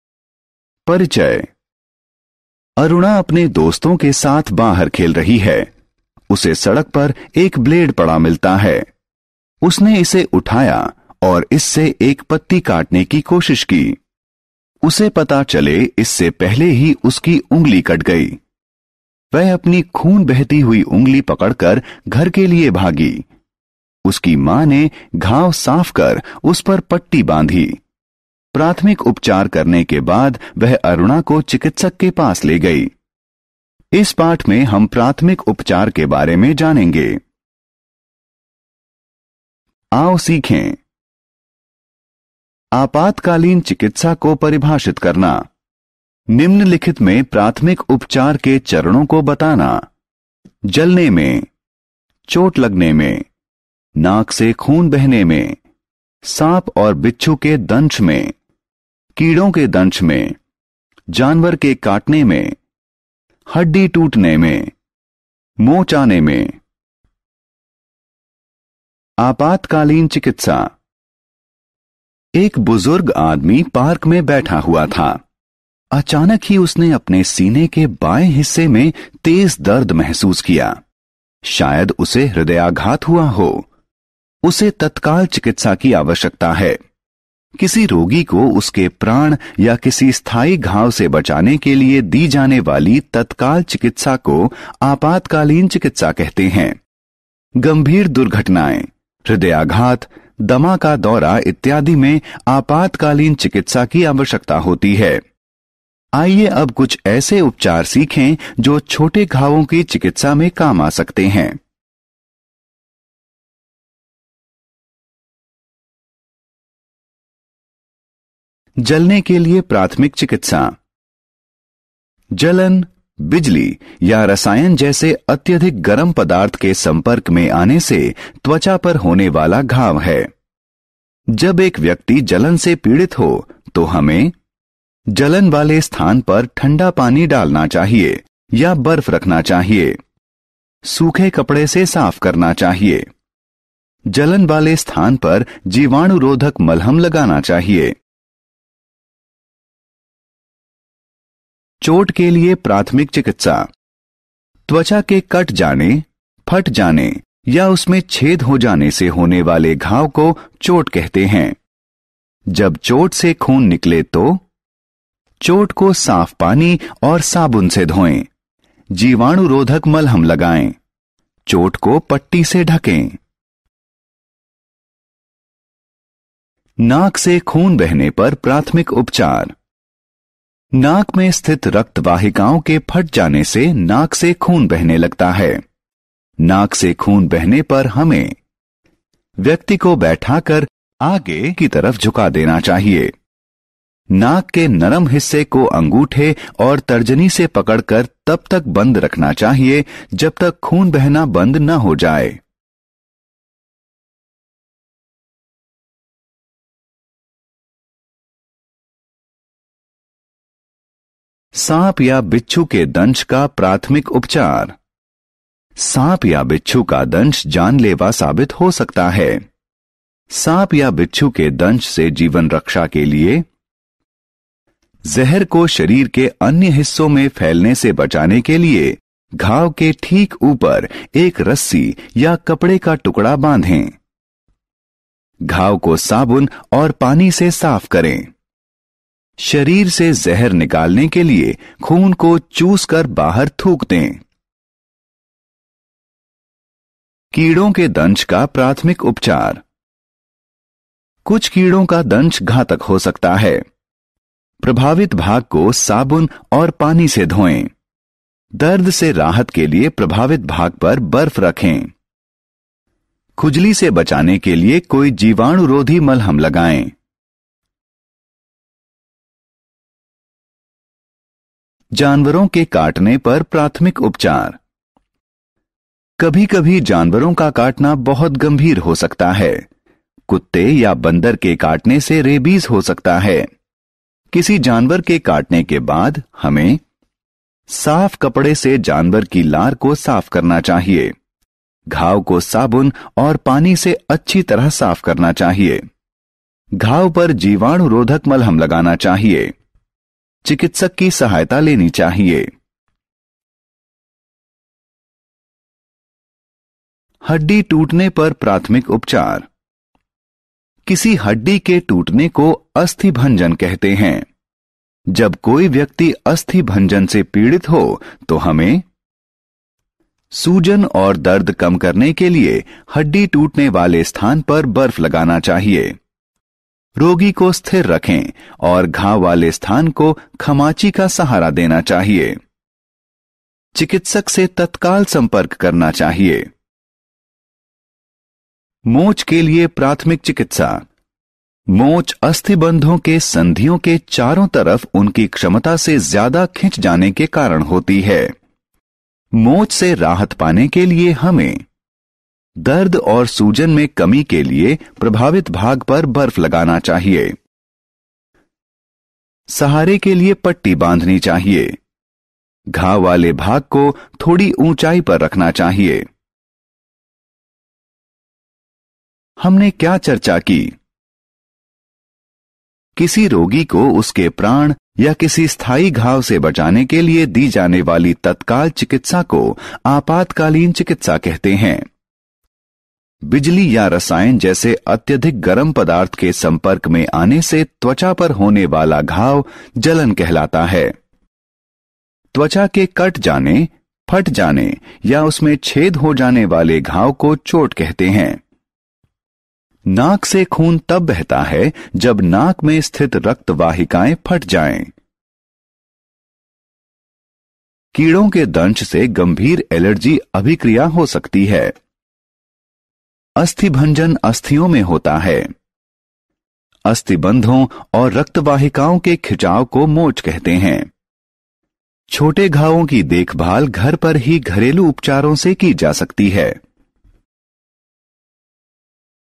4th- science hindi- Emergency Care And Home Remedies
In this video, we will teach students about basic emergency care and home remedies that can be used to treat minor injuries and illnesses. We will start by explaining what emergency care means and the importance of being prepared for emergencies.

Next, we will explore different types of emergencies that can occur at home, school, or in public places, such as cuts, bruises, burns, and insect bites. We will discuss how to identify the severity of an injury or illness and when it is necessary to seek professional medical help.

We will also introduce students to various home remedies that can be used to treat minor injuries and illnesses, such as applying ice packs to reduce swelling or using natural remedies to treat common colds and flu.